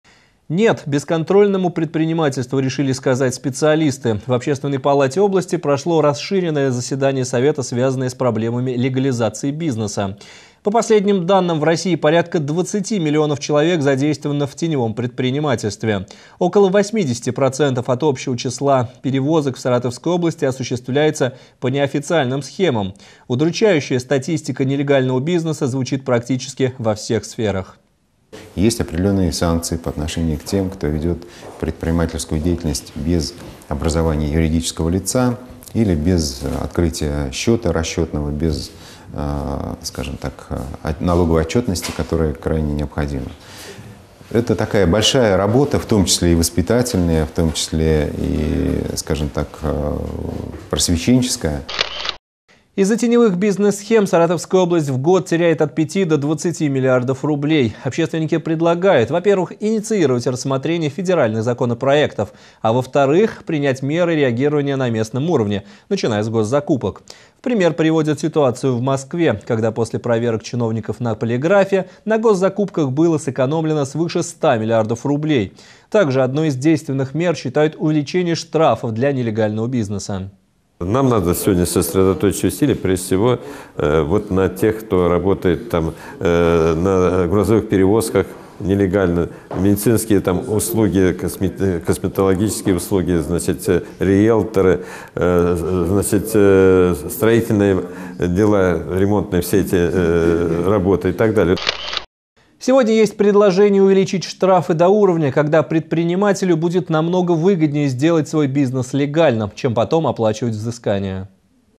В Саратове специалисты решили бороться с теневым бизнесом
В общественной палате области прошло расширенное заседание Совета, связанное с проблемами легализации бизнеса. По последним данным, в России порядка 20 миллионов человек задействованы в теневом предпринимательстве. Сюжет вышел в эфир 19 декабря.